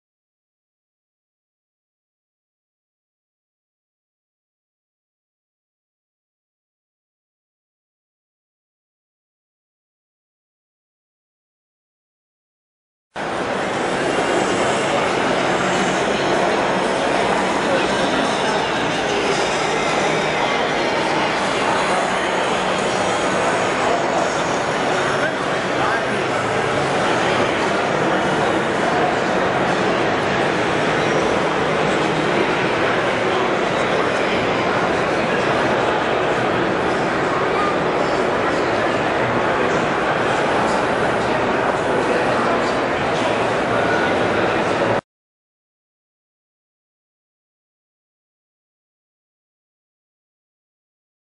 2013 Harley Davidson FXDWG Wide Glide
Footage from the 2013 Toledo Auto Show